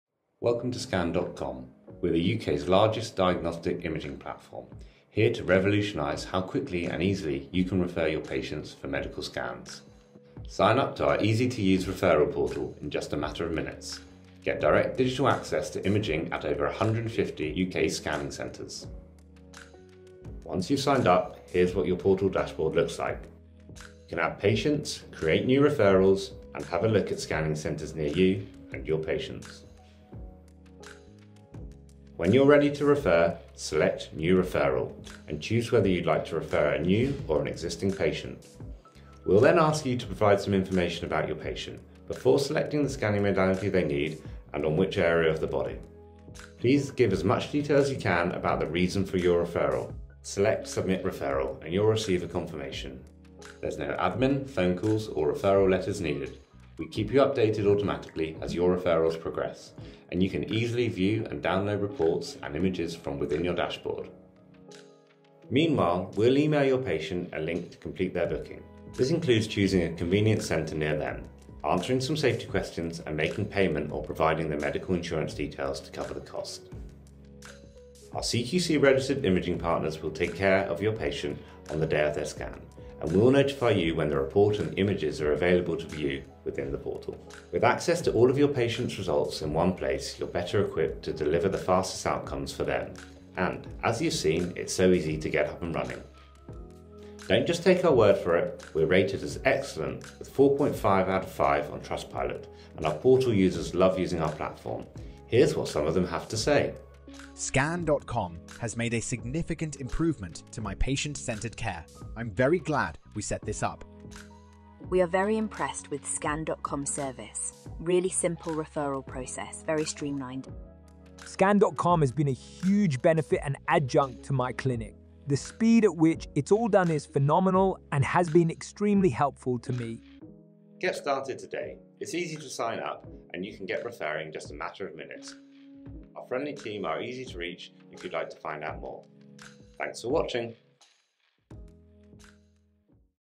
Scan.com Portal Walkthrough - Demo & Features
Scan.com's referral portal makes it easy to arrange medical imaging scans, such as MRI and CT, all in one place.

In this demo, find out how to refer your patients through our free-to -use platform, and get digital access to results and images. With next-day appointments, fast results, and a database to manage all of your imaging, Scan.com's portal slots seamlessly into your existing workflows.